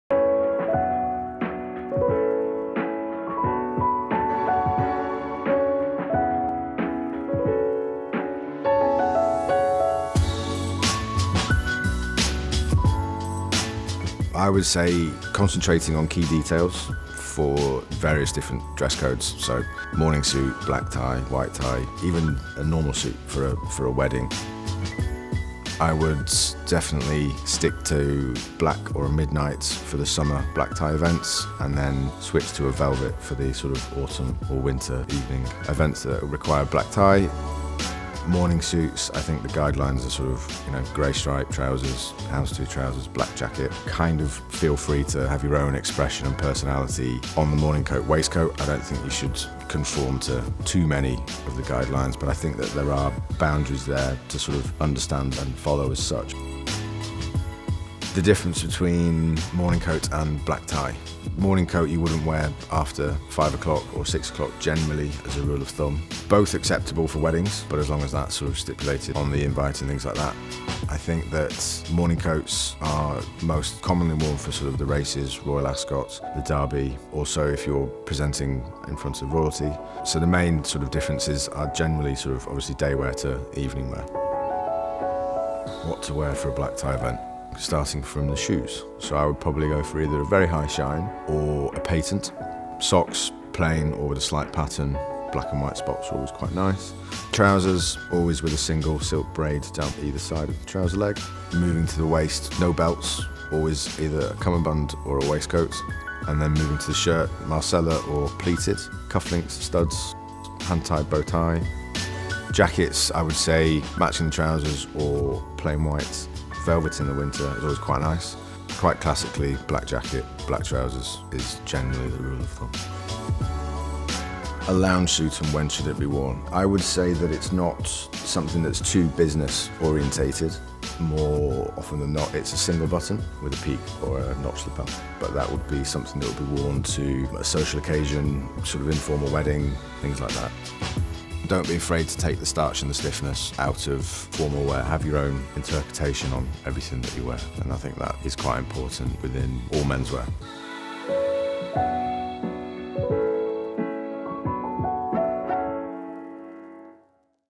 Men's Guide to Dress Codes with Tim Lord / Bicester Village and Debrett's
Debrett’s and Bicester Village “Guide to British Style” bring you men's guide to dress codes, in an interview with stylist and etiquette expert Tim Lord within the beautiful grounds of Blenheim Palace.

Deciphering dress codes for daytime and evening events requiring black tie or morning dress isn't easy.

Morning dress:
high shine shoes, red tie, white shirt and brogues by Charles Tyrwhitt